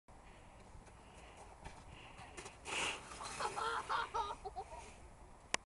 run down
board so i ran down a hill and wipped out i no dumb